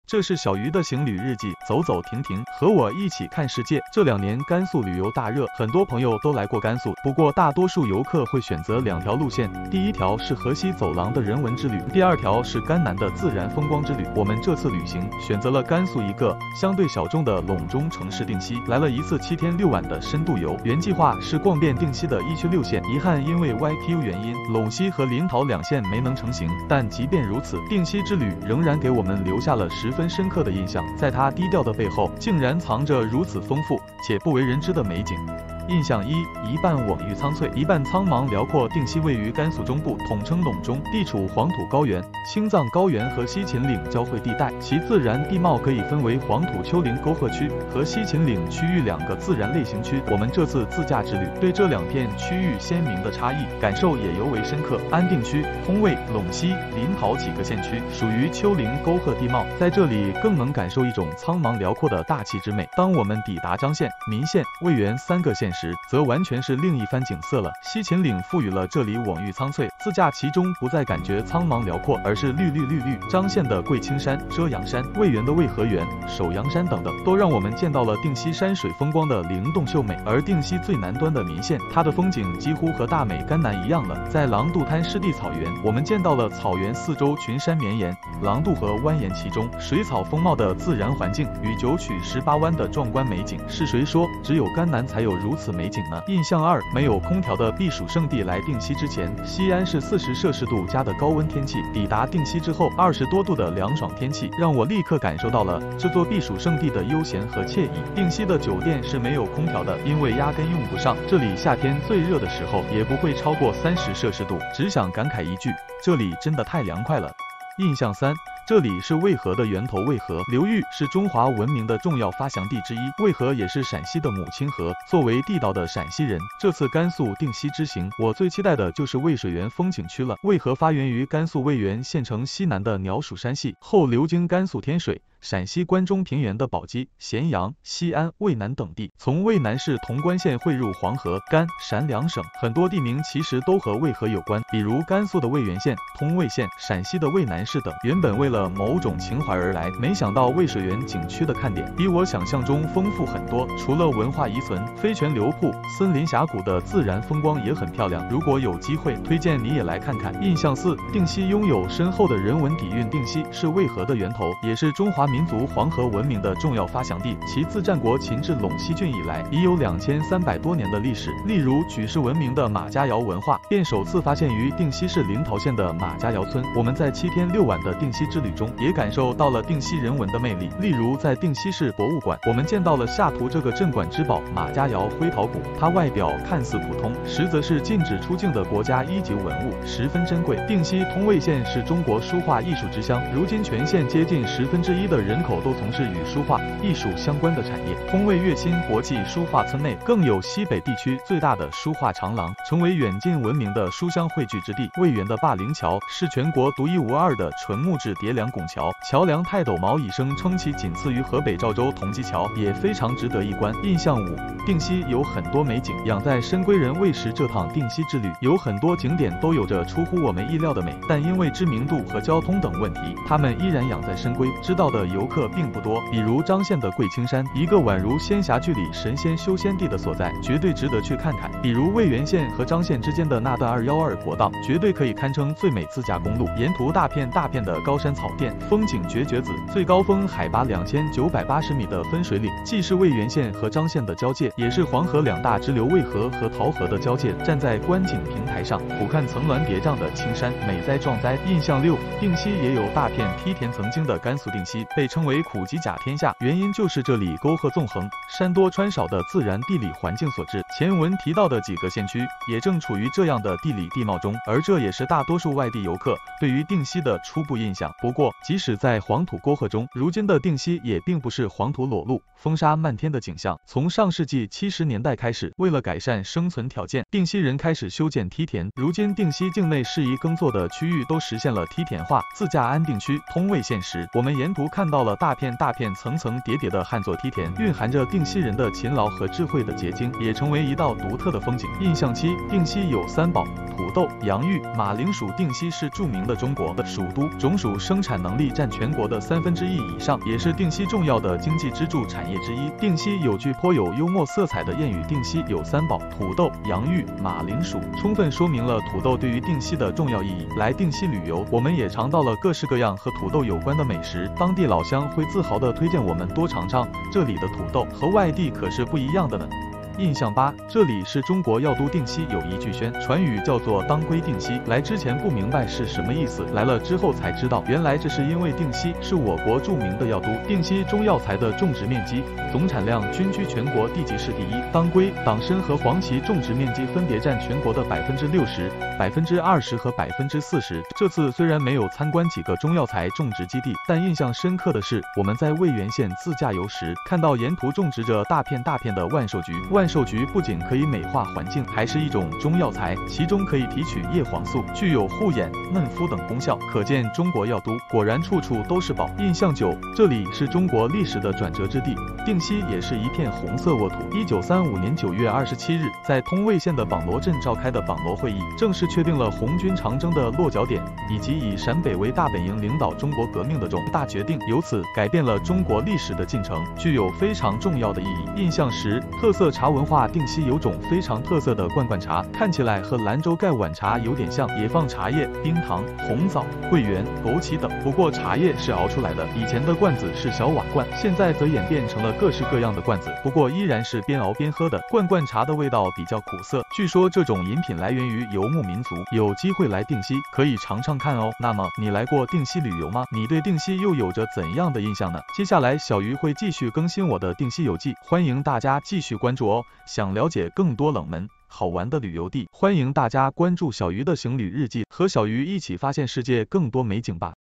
西安游客第一次去甘肃定西，10个初印象：许多都颠覆想象
印象五：定西有很多美景，养在深闺人未识这趟定西之旅，有很多景点都有着出乎我们意料的美。